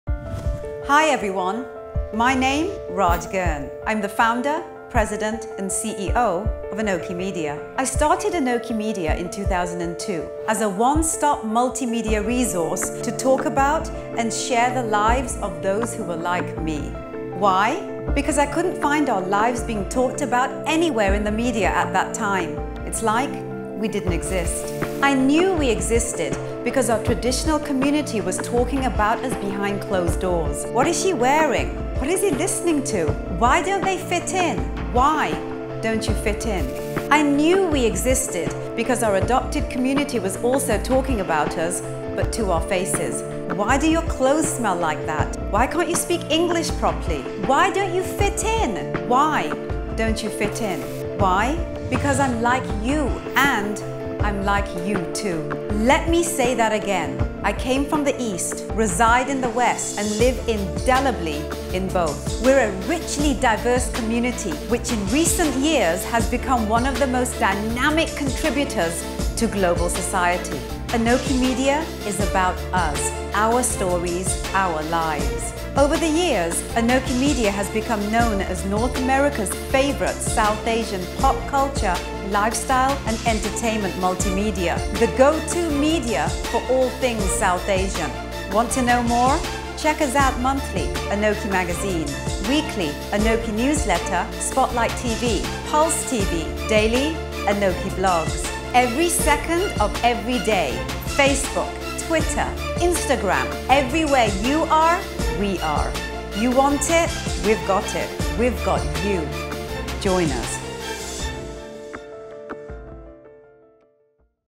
FOUNDER SHARES WHY ANOKHI MEDIA MATTERS TODAY
ANOKHI MEDIA's Founder, President & CEO, Raj Girn shares the story of why she decided to launch ANOKHI MEDIA 12 years ago at a time when the South Asian community was under the mainstream radar.  She talks about the parallel journey of the global South Asian community and the growth of the ANOKHI MEDIA brand after its inception in 2002. Today, it has grown internationally as a one-stop, multimedia resource to talk about and share the lives of today’s dynamic, cosmopolitan South Asian.

In her words: "We came from the East, reside in the West and live indelibly in both! We are a richly diverse community, which in recent years has become one of the most dynamic contributors to global society".

Watch Raj's video message here to learn more about why ANOKHI MEDIA matters today!